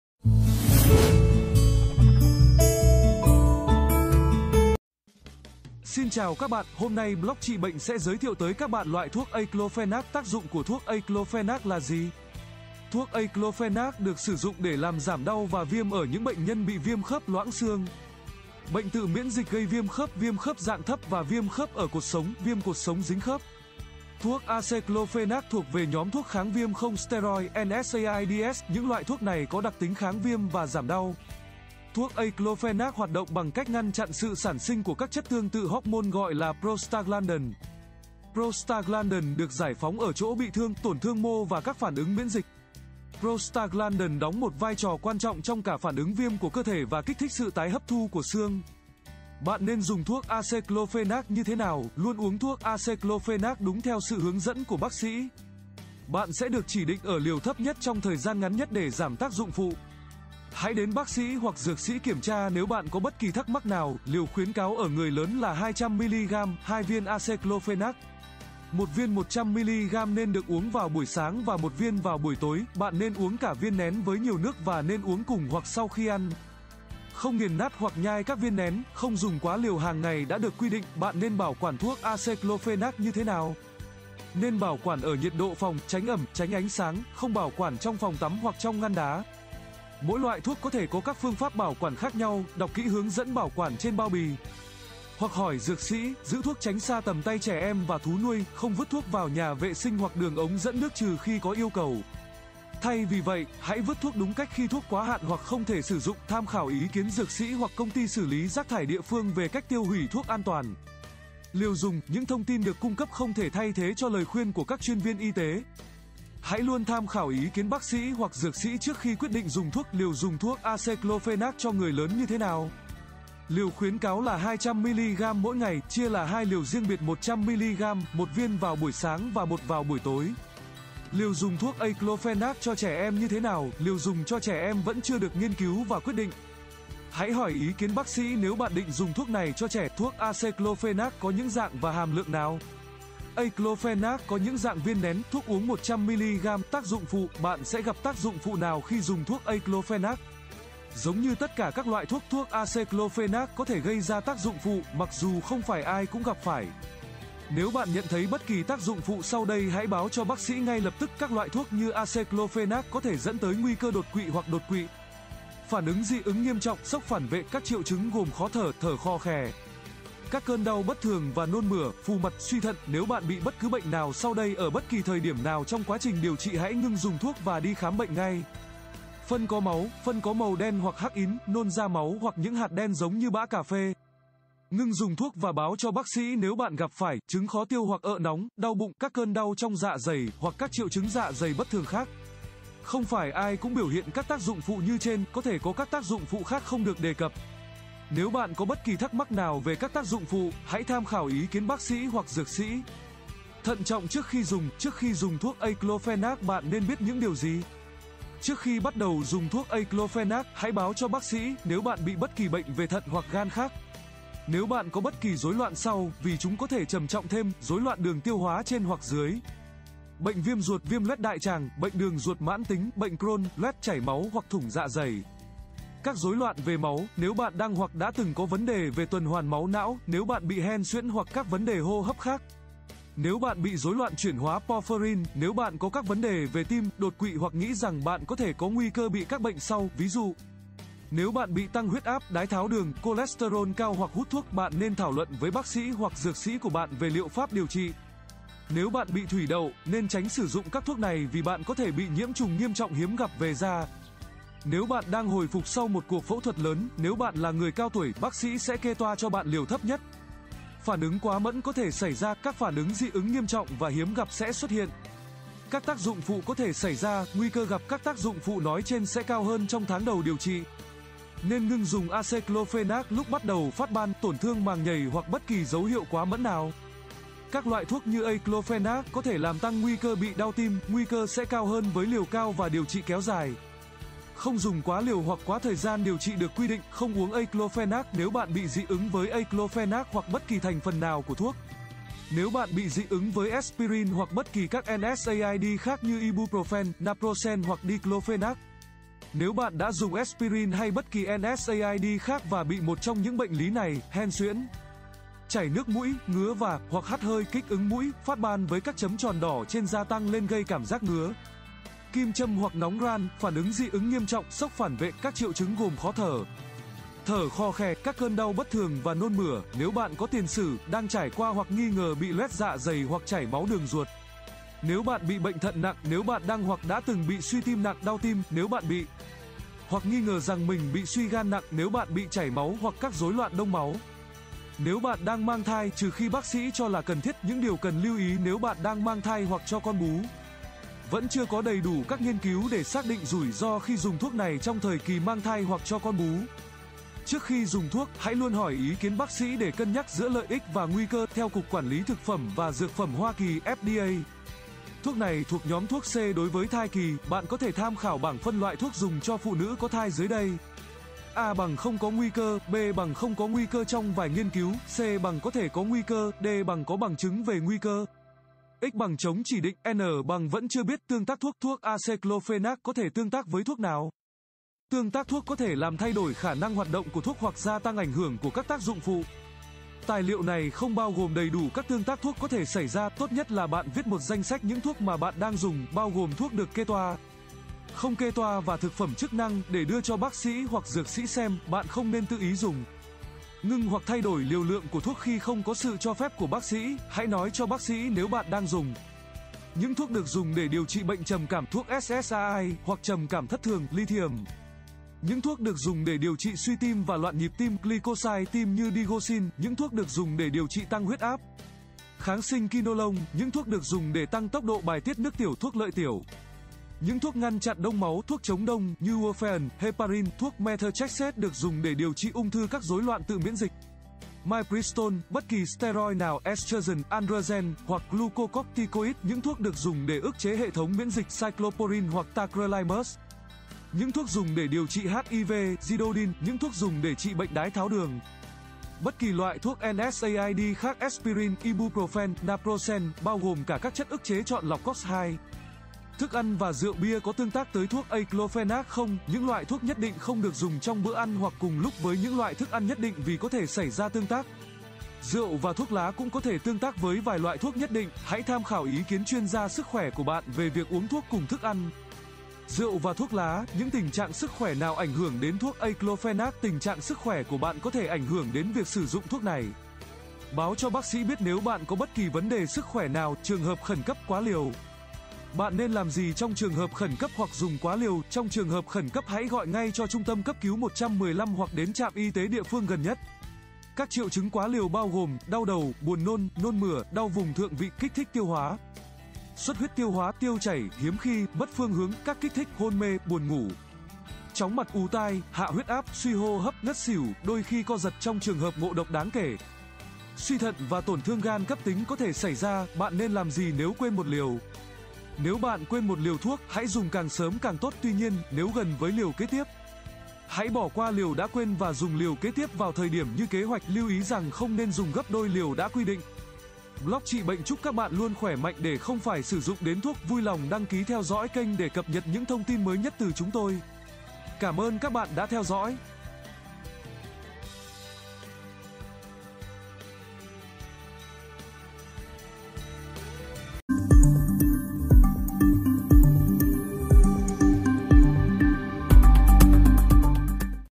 Thuốc Aceclofenac
Tác dụng của thuốc aceclofenac là gì?

Thuốc aceclofenac được sử dụng để làm giảm đau và viêm ở những bệnh nhân bị viêm khớp (loãng xương), bệnh tự miễn dịch gây viêm khớp (viêm khớp dạng thấp) và viêm khớp ở cột sống (viêm cột sống dính khớp).

Thuốc aceclofenac thuộc về nhóm thuốc kháng viêm không steroid (NSAIDs). Những loại thuốc này có đặc tính kháng viêm và giảm đau.

Thuốc aceclofenac hoạt động bằng cách ngăn chặn sự sản sinh của các chất tương tự hormone gọi là prostaglandin. Prostaglandin được giải phóng ở chỗ bị thương, tổn thương mô và các phản ứng miễn dịch. Prostaglandin đóng một vai trò quan trọng trong cả phản ứng viêm của cơ thể và kích thích sự tái hấp thu của xương.

Bạn nên dùng thuốc aceclofenac như thế nào?
Luôn uống thuốc aceclofenac đúng theo sự hướng dẫn của bác sĩ. Bạn sẽ được chỉ định ở liều thấp nhất trong thời gian ngắn nhất để giảm tác dụng phụ. Hãy đến bác sĩ hoặc dược sĩ kiểm tra nếu bạn có bất kỳ thắc mắc nào.

Liều khuyến cáo ở người lớn là 200 mg (hai viên aceclofenac). Một viên 100 mg nên được uống vào buổi sáng và một viên vào buổi tối.

Bạn nên uống cả viên nén với nhiều nước và nên uống cùng hoặc sau khi ăn. Không nghiền nát hoặc nhai các viên nén.

Không dùng quá liều hàng ngày đã được quy định.

Bạn nên bảo quản thuốc aceclofenac như thế nào?
Nên bảo quản ở nhiệt độ phòng, tránh ẩm, tránh ánh sáng. Không bảo quản trong phòng tắm hoặc trong ngăn đá. Mỗi loại thuốc có thể có các phương pháp bảo quản khác nhau. Đọc kỹ hướng dẫn bảo quản trên bao bì, hoặc hỏi dược sĩ. Giữ thuốc tránh xa tầm tay trẻ em và thú nuôi.

Không vứt thuốc vào nhà vệ sinh hoặc đường ống dẫn nước trừ khi có yêu cầu. Thay vì vậy, hãy vứt thuốc đúng cách khi thuốc quá hạn hoặc không thể sử dụng. Tham khảo ý kiến dược sĩ hoặc công ty xử lý rác thải địa phương về cách tiêu hủy thuốc an toàn.

Liều dùng
Những thông tin được cung cấp không thể thay thế cho lời khuyên của các chuyên viên y tế. Hãy luôn tham khảo ý kiến bác sĩ hoặc dược sĩ trước khi quyết định dùng thuốc.

Liều dùng thuốc aceclofenac cho người lớn như thế nào?
Liều khuyến cáo là 200 mg mỗi ngày, chia là hai liều riêng biệt 100 mg, một viên vào buổi sáng và một vào buổi tối.

Liều dùng thuốc aceclofenac cho trẻ em như thế nào?
Liều dùng cho trẻ em vẫn chưa được nghiên cứu và quyết định. Hãy hỏi ý kiến bác sĩ nếu bạn định dùng thuốc này cho trẻ.

Thuốc aceclofenac có những dạng và hàm lượng nào?
Aceclofenac có những dạng viên nén, thuốc uống 100 mg.